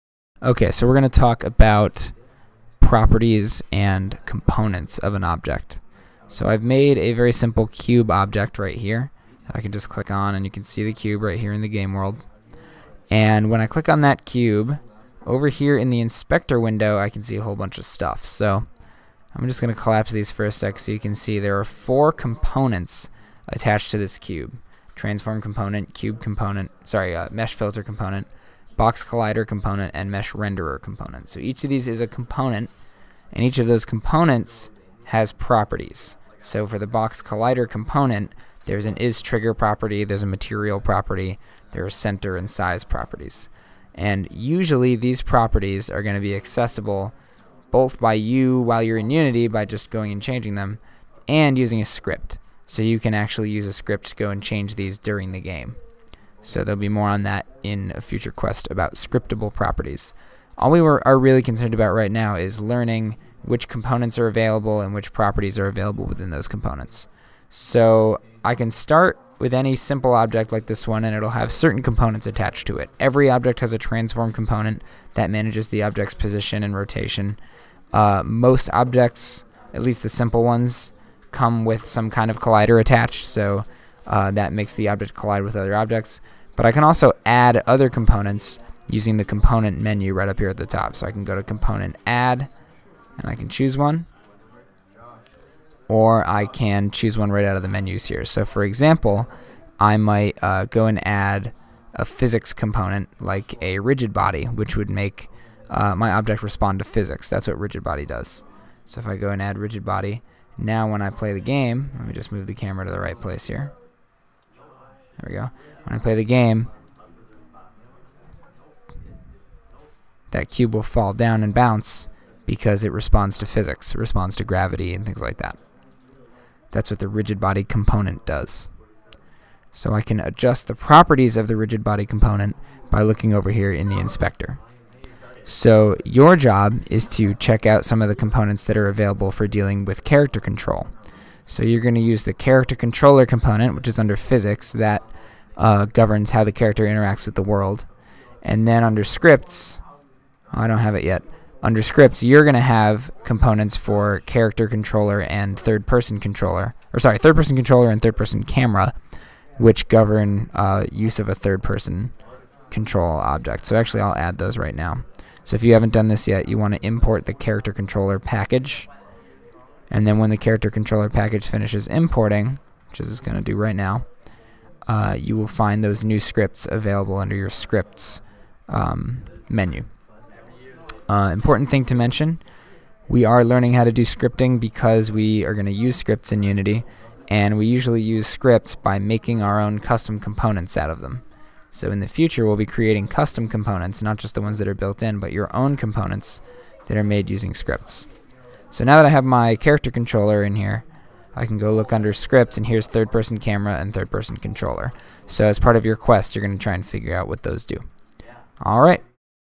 Components and Properties in Unity 3D
This video explains what components and properties are in Unity.